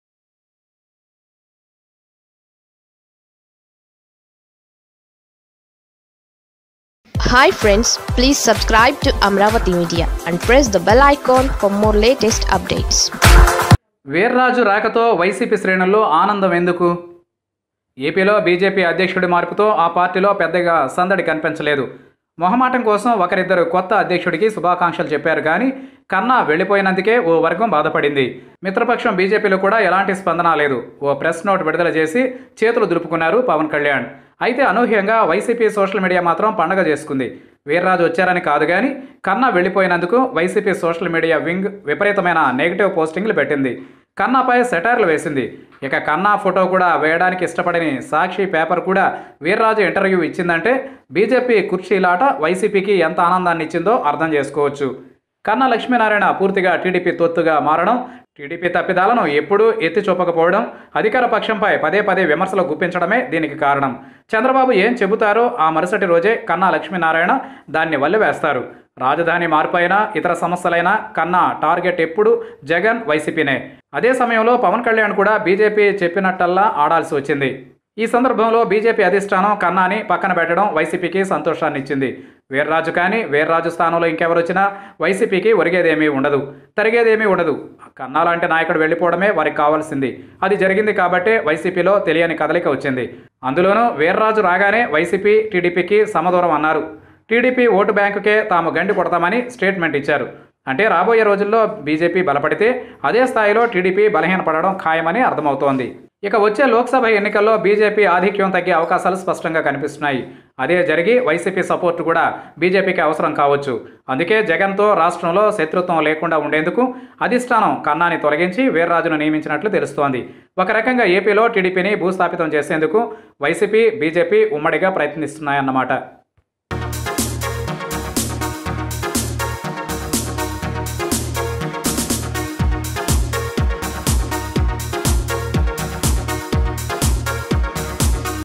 సోము వీర్రాజు ఎంట్రీతో వైసీపీ ఫుల్ హ్యాపీ..ఎందుకంటే?| Why YSRCP Cadre Happy with Somu Veerraju Post?
YSRCP Cadre are more happy than BJP activists with Somu Veerraju being appointed AP BJP president. There is a big reason behind this.